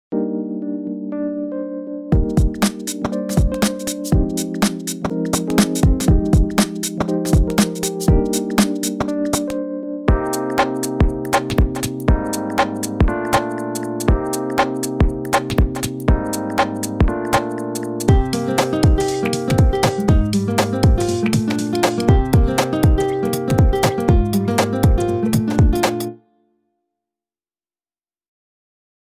Karan Goel Royalty Free Music Bundle Sample
51 Royalty Free Music Tracks from Karan Goel.  These music tracks can be added to your videos, websites, products or work done for clients.  Comes with Private Label Rights (PLR). This is a sample clip.  Just one of over 150 Items included in the Nicheprof's 65th Birthday Bonanza.  to keep up with the other items added between now and August 20, 2012